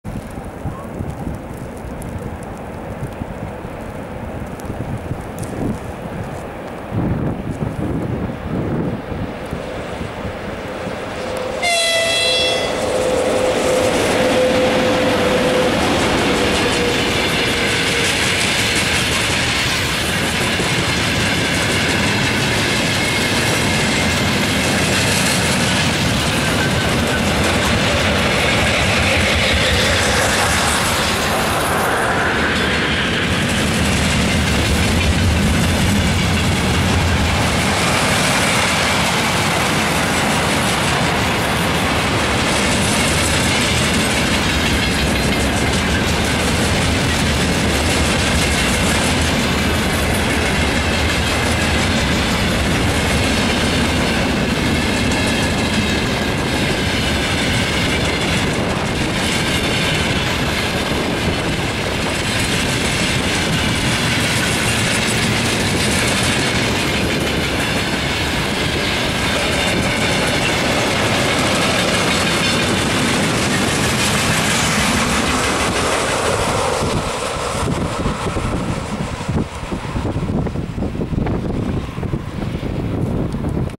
ВЛ80С-412 с грузовым поездом, перегон Кама - Шолья
Электровоз ВЛ80С-412 с грузовым поездом следует по перегону Кама - Шолья
Россия/Удмуртия
24.02.20